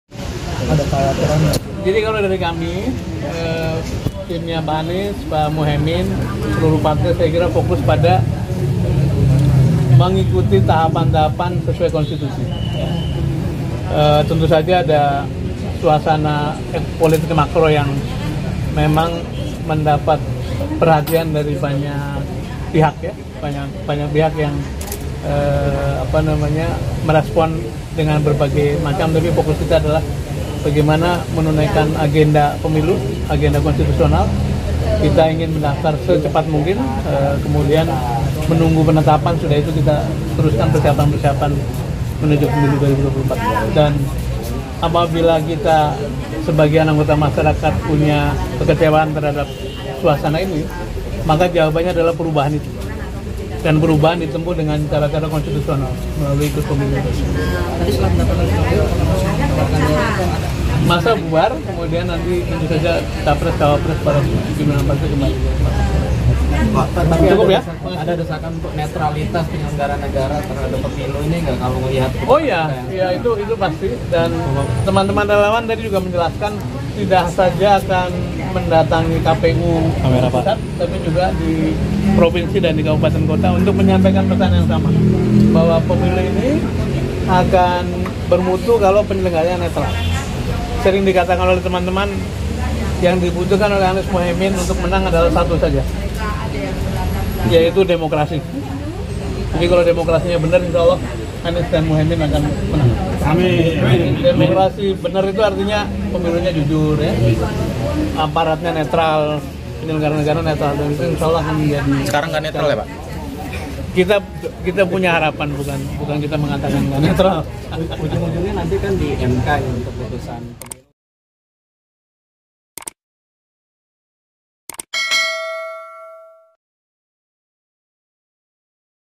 Sudirman Said: Kalau Demokrasi Benar, Anies-Muhaimin Akan Menang
Juru Bicara Anies Baswedan Sudirman Said mengatakan, pasangan Anies-Muhaimin Iskandar bisa saja memenangkan Pemilu jika penyelenggaraannya berlangsung dengan netral, Selasa (17/10/2023).

Sudirman mengatakan, pasangan Anies-Muhaimin hanya membutuhkan demokrasi yang netral jika ingin memenangkan Pemilu 2024.

"Sering dikatakan oleh teman-teman yang dibutuhkan oleh Anies-Muhaimin untuk menang satu saja yaitu demokrasi. Mungkin kalau demokrasinya benar, InsyaAllah Anies dan Muhaimin akan menang," ujarnya Sudirman di Rumah Koalisi Perubahan, Jakarta Selatan, Selasa (17/10/2023).

Video Jurnalis: Syalutan Ilham
Penulis Naskah: Syalutan Ilham
Video Editor: Syalutan Ilham
Produser: Okky Mahdi Yasser